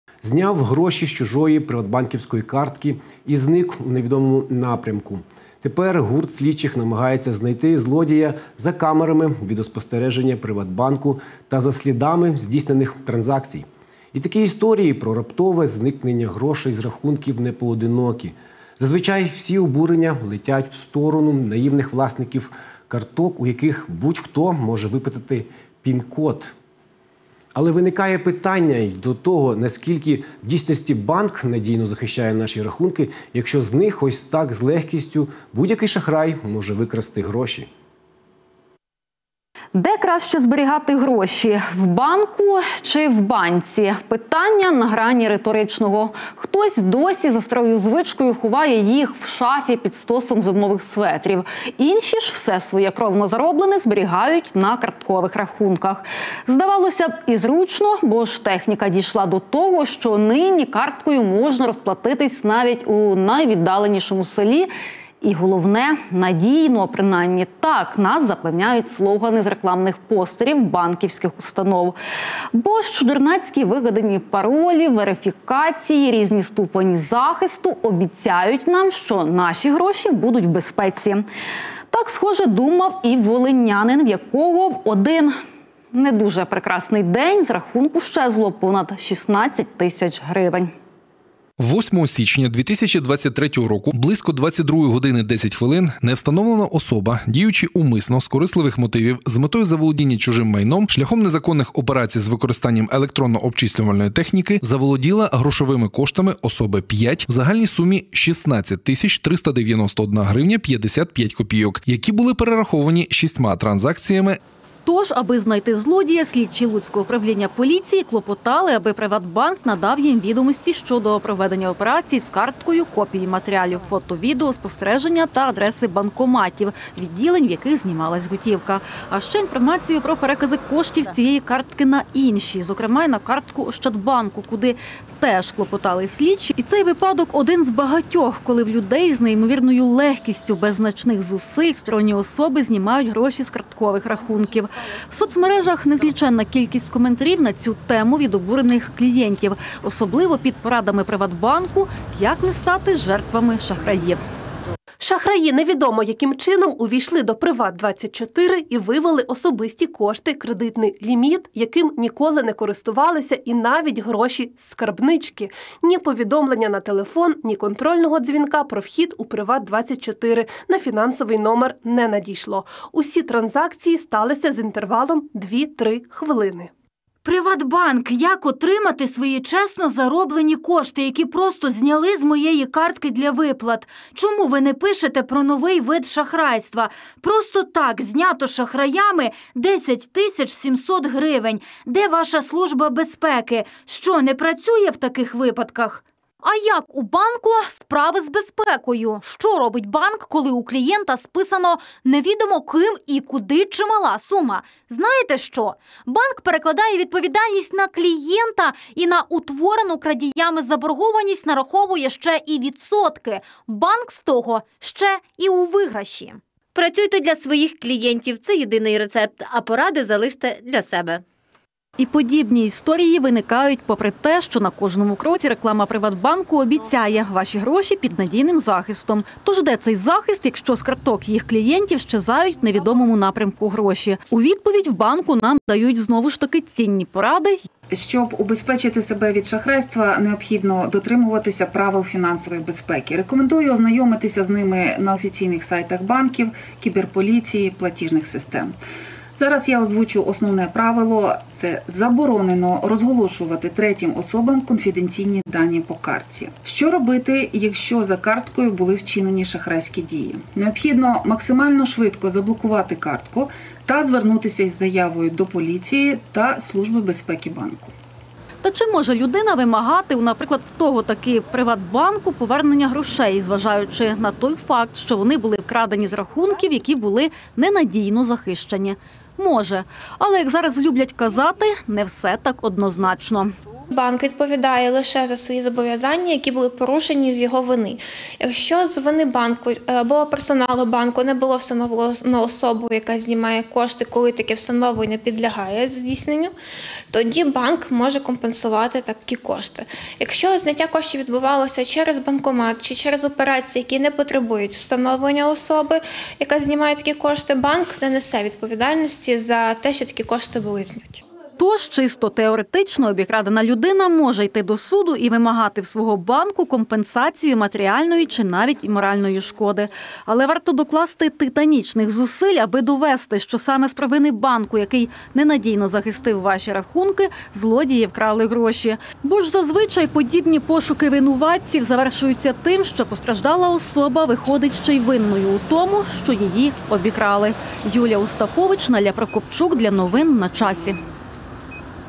Гроші – Ваші, та лежать у банку: хто поверне вкрадене шахраями?
Зняв гроші з чужої приватбанківської картки - і зник у невідомому напрямку. Тепер гурт слідчих намагається знайти злодія за камерами відеоспостережень «ПриватБанку» та за слідами здійснених трансакцій. І такі історії, про раптове зникнення грошей з рахунків – непоодинокі. Зазвичай всі обурення летять в сторону наївних власників карток, у яких будь-хто може випитати пін-код. Але виникає питання й до того, наскільки в дійсності банк надійно захищає наші рахунки, якщо з них ось так з легкістю будь-який шахрай може вкрасти гроші?